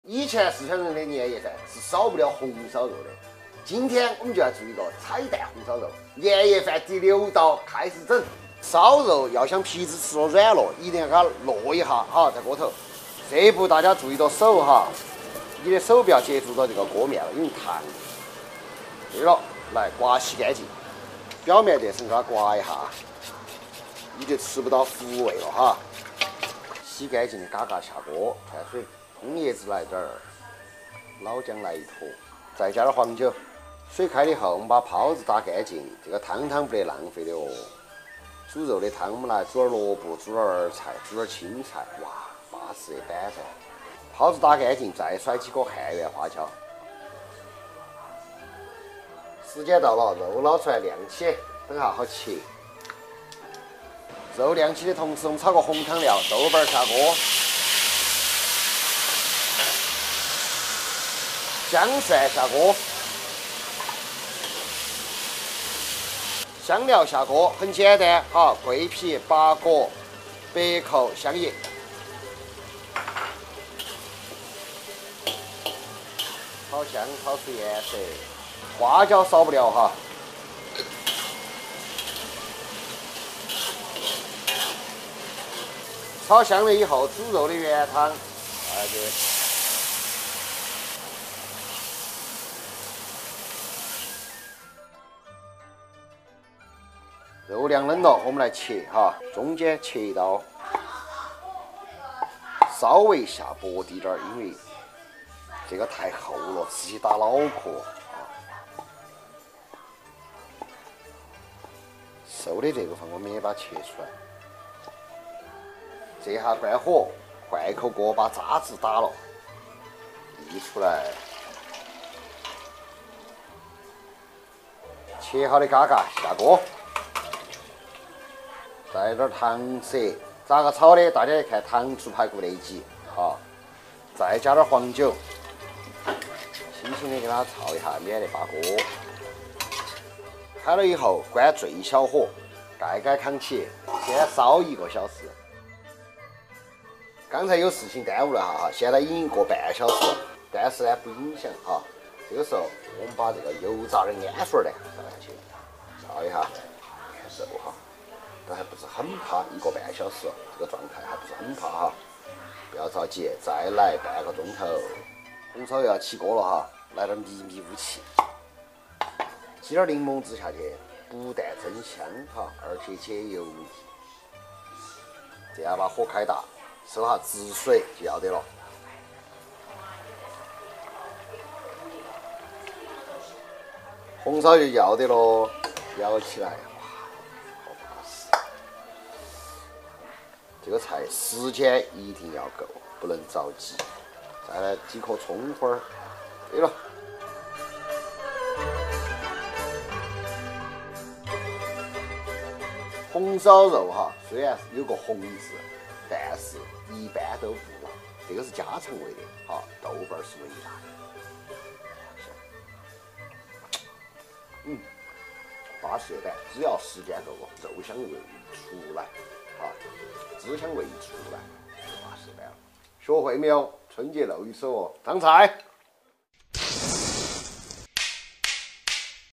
【火筵家常】年夜饭第六道彩蛋红烧肉，加入神秘食材解腻还不油，不等看完就饿
喜欢我们的频道，别忘了点赞，评论，分享。
下期想看火哥做什么菜，请在下方留言。